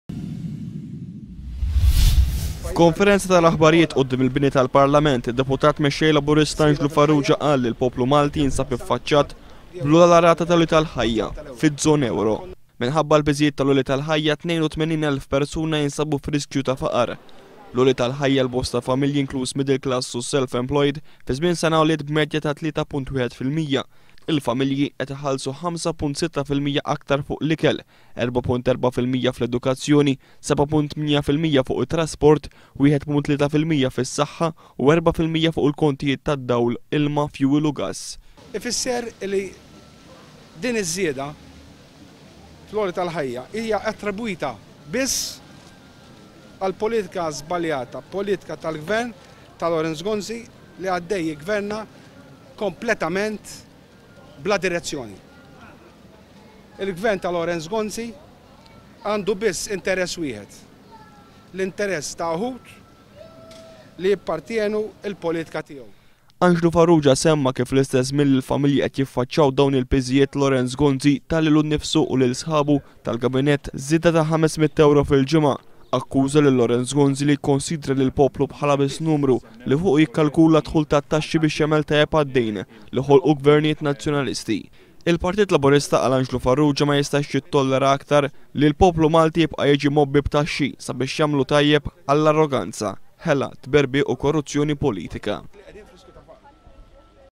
Malta bl-għola żieda fl-għoli tal-ħajja fiż-żona Ewro.
Id-Deputat Mexxej Laburista Anglu Farrugia qal li l-gholi tal-hajja qed jghakkes il-klassi tal-haddiema b'salarju, l-pensjonanti u dawk vulnerabbli li jinsabu f'riskju ta' faqar.Matul ix-xahar ta' Gunju, pajjizna rregistra l-oghla zieda fl-gholi tal-hajja fiz-zona Ewro.